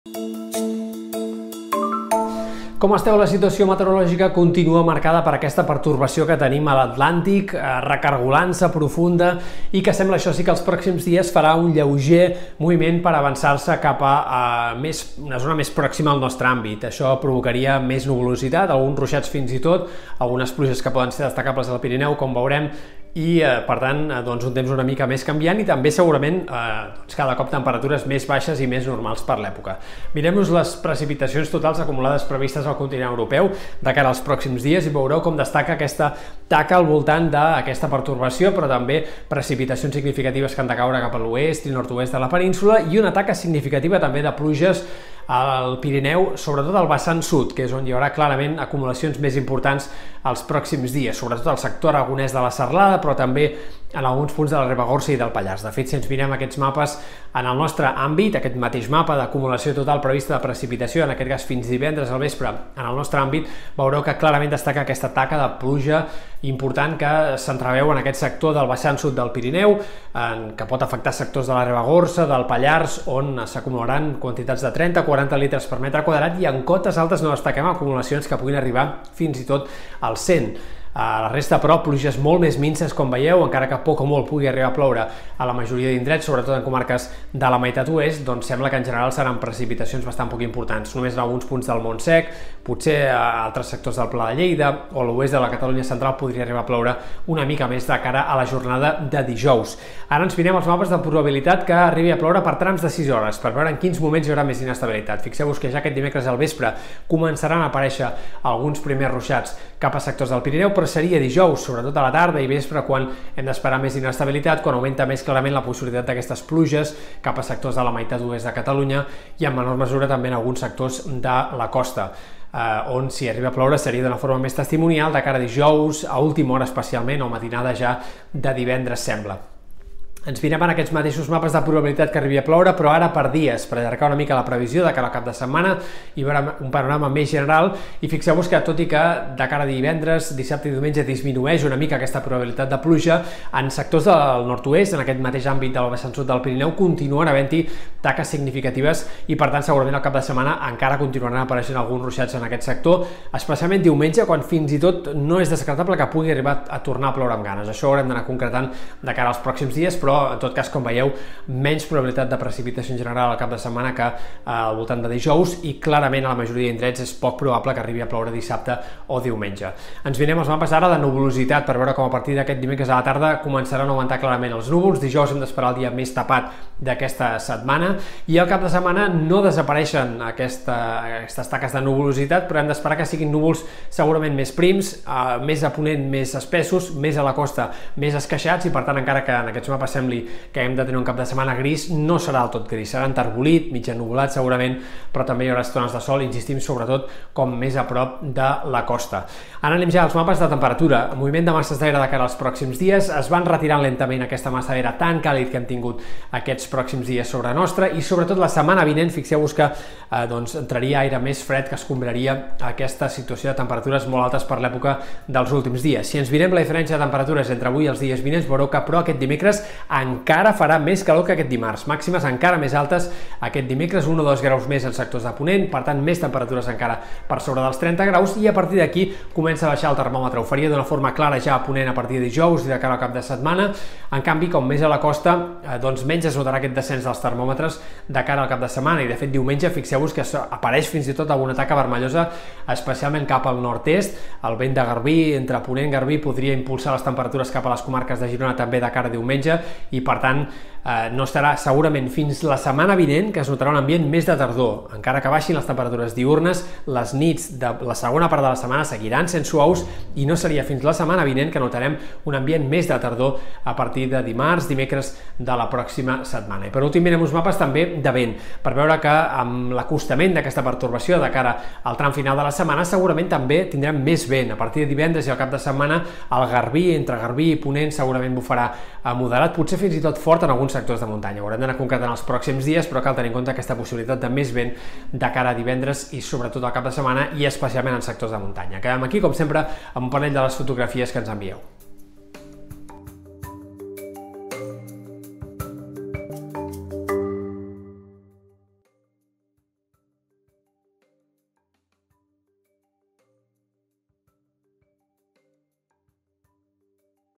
La méteo: un canvi de temps apagarà la calor d'estiu a partir de dijous
Demà les màximes encara podrien ser una mica més altes que avui a Ponent.